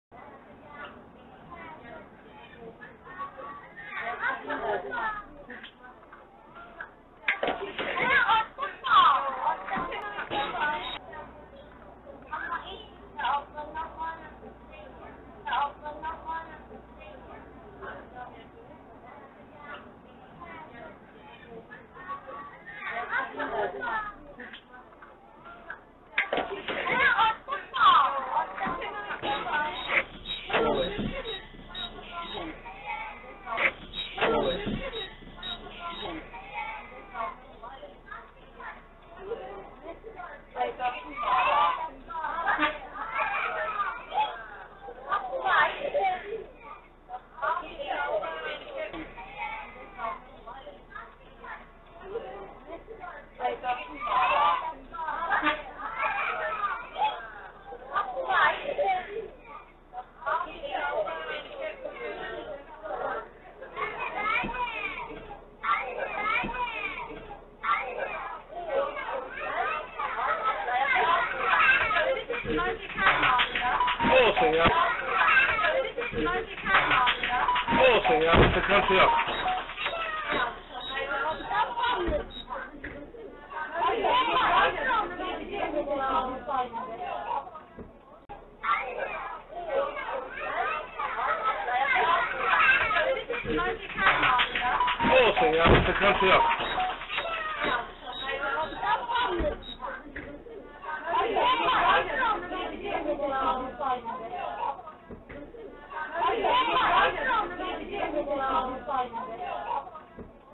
Otobüs şoföründen alkışlanacak hareket
Kocaeli Büyükşehir Belediyesi UlaşımPark şoförü, otobüsle seyir halindeyken yol üstünde gördüğü kaplumbağayı ezilmekten kurtararak güvenli bölgeye götürdü. O anlar araç kamerasına yansırken, şoförün hareketi takdir topladı.